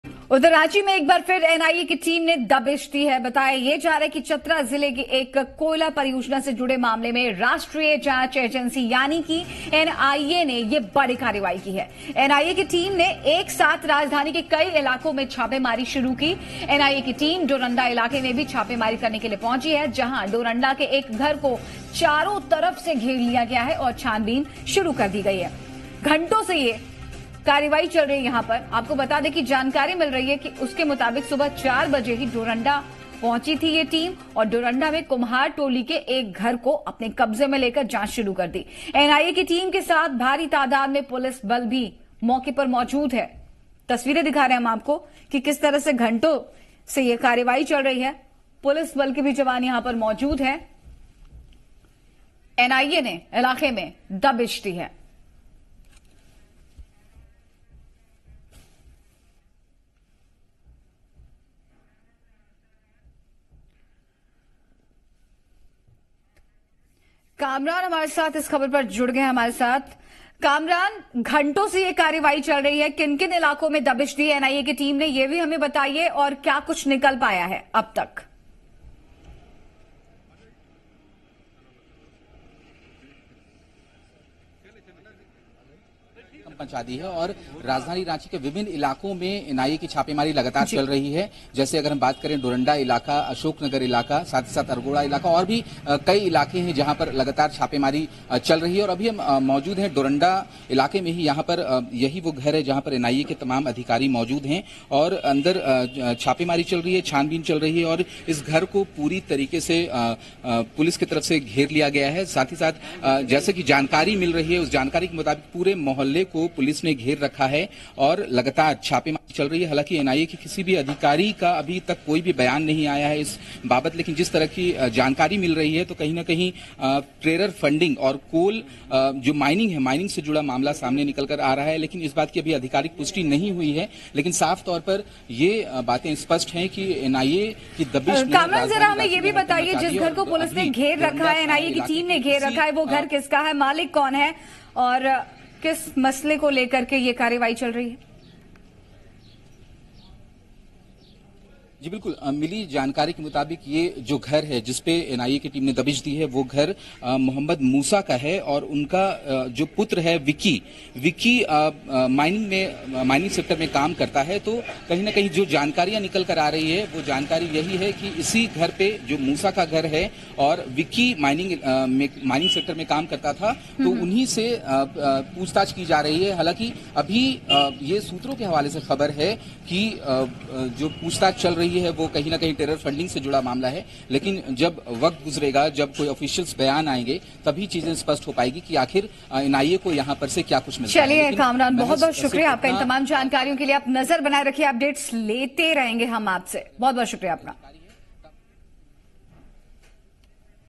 रांची: टेरर फंडिंग के खिलाफ राजधानी में NIA का बड़ा एक्शन
टेरर फंडिंग मामले में एनआईए की टीम ने रांची में दबिश दी है. एनआईए की टीम ने एक साथ राजधानी के कई इलाके में छापेमारी की. डोरंडा इलाके एक घर में एनआईए की टीम ने कई घंटों जांच की है.